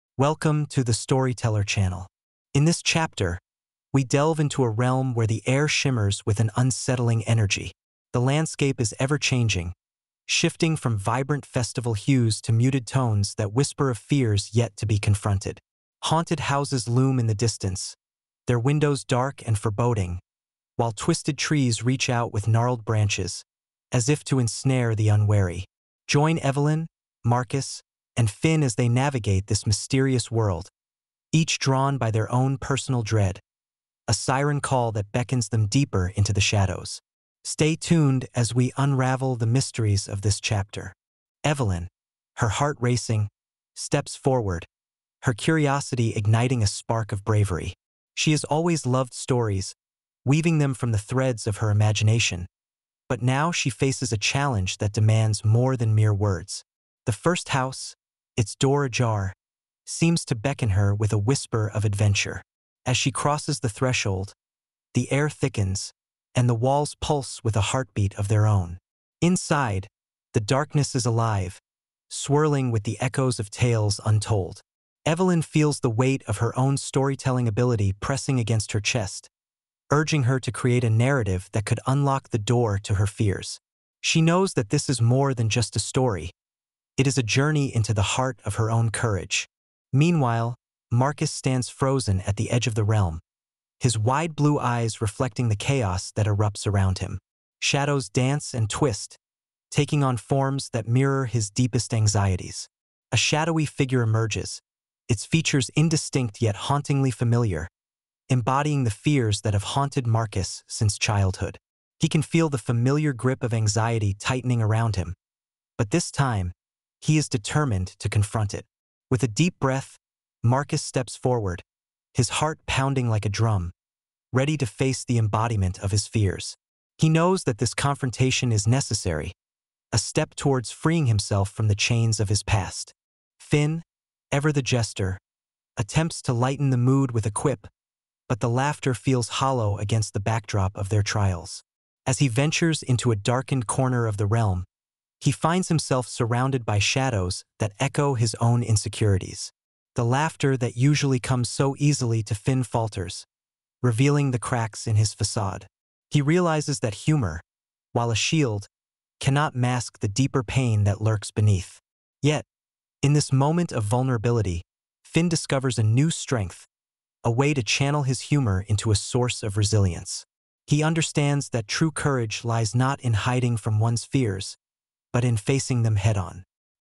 Halloween Whispers [Year 2024]
In this chapter, we delve into a realm where the air shimmers with an unsettling energy. The landscape is ever-changing, shifting from vibrant festival hues to muted tones that whisper of fears yet to be confronted. Haunted houses loom in the distance, their windows dark and foreboding, while twisted trees reach out with gnarled branches, as if to ensnare the unwary. Join Evelyn, Marcus, and Finn as they navigate this mysterious world, each drawn by their own personal dread, a siren call that beckons them deeper into the shadows.